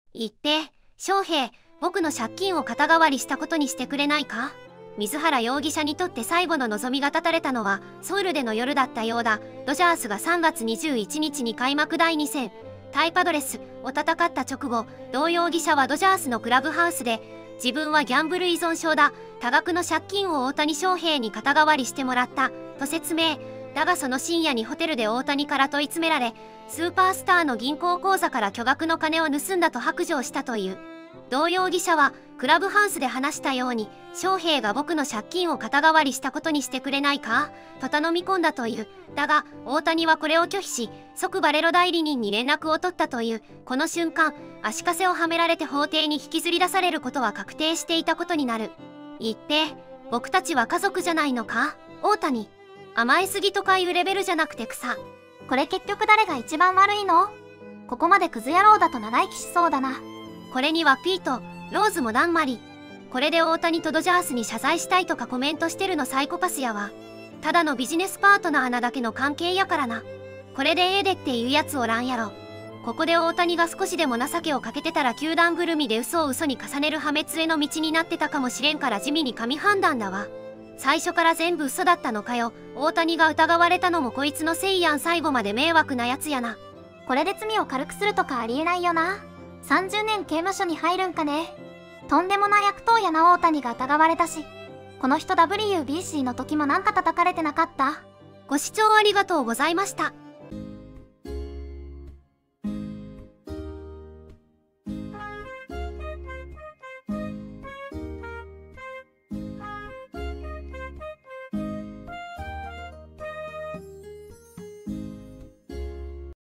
【大谷「無理」】一平「翔平 僕の借金を肩代わりしたことにしてくれないか？」【プロ野球反応集】【2chスレ】【1分動画】【5chスレ】
LAMP BGM